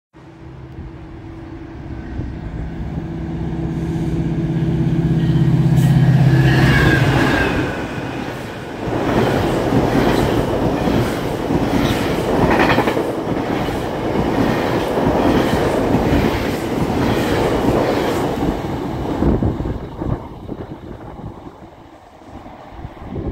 (Equipment Move) GO E-1132 - 639 With 315 At Bronte
(Equipment Move)

Originated: Appleby GO Station

Destination: Oakville GO Station

(Service Operation)

Originates: Oakville GO Station

Terminates: Toronto Union Station